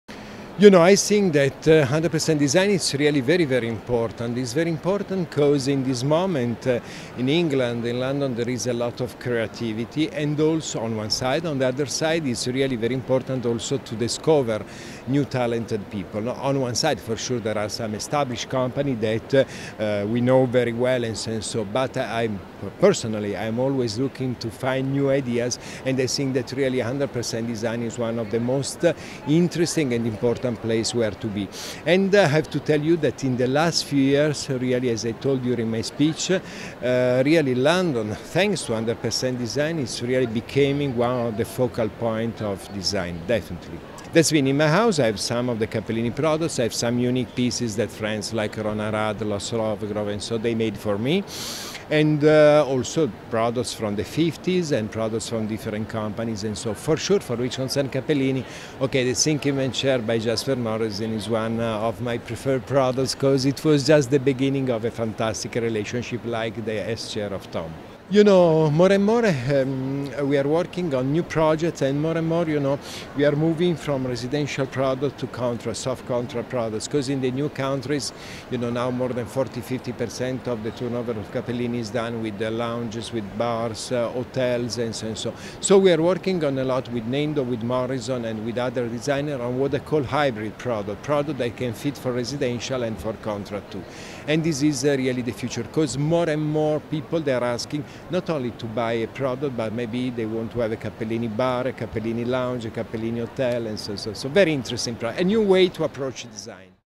Giulio Cappellini at 100% Design 2013
Leading global designer Giulio Cappellini tells us why 100% Design and London itself is so important to the design and architecture industry.